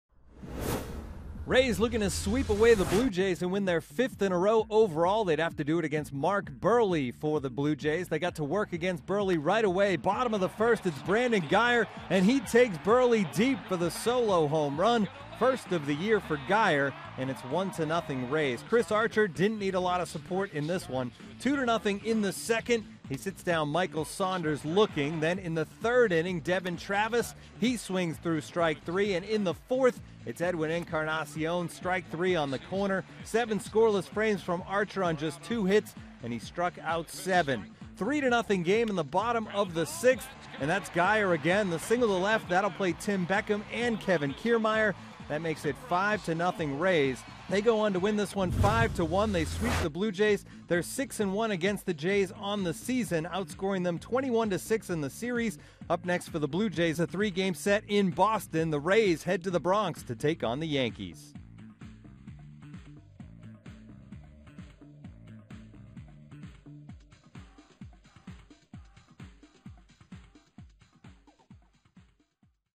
4/26/15: Archer, Guyer power Rays past Blue Jays
Daily Recap: Chris Archer tossed seven shutout innings as Brandon Guyer homered and drove in a career-high four runs in the Rays' 5-1 win